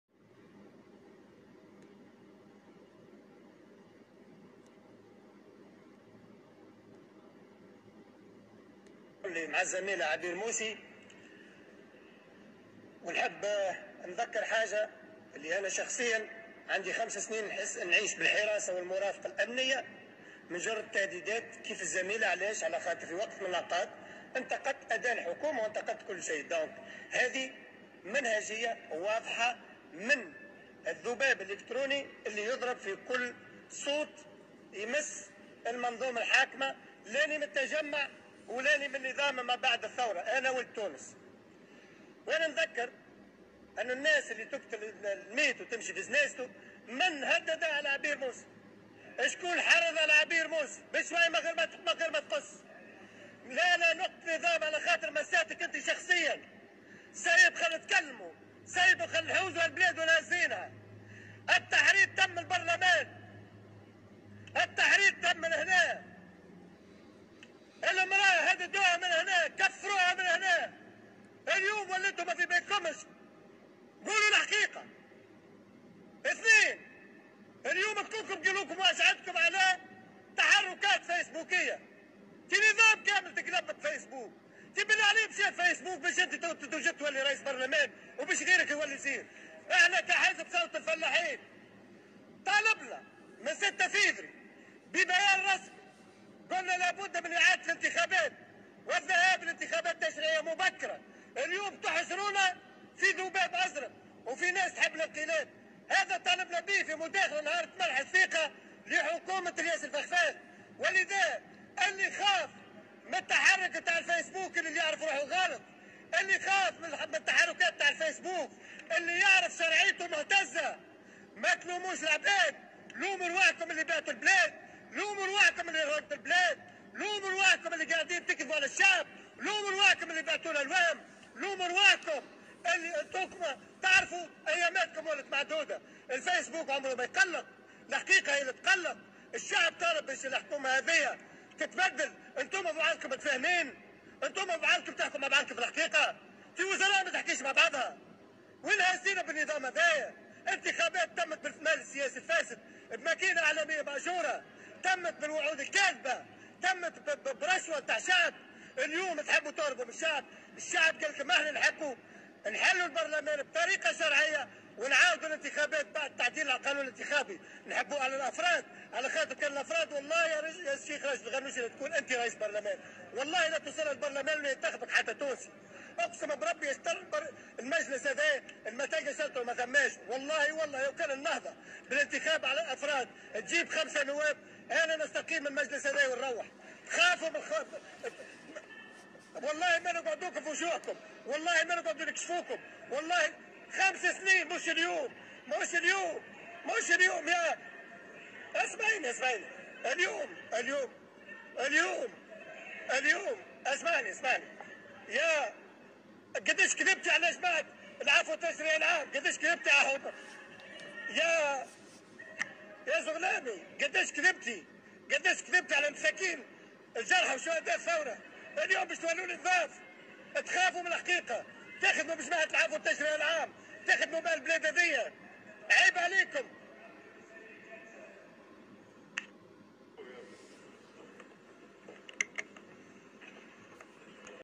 مجلس الشعب آلتونسي النائب فيصل التبيني يعلن تضامنه مع عبير موسي
النائب فيصل التبيني يتضامن مع عبير موسي و يتهم الغنوشي و النهضة بالتحريض على كل من يعارضهم و يختلف معهم.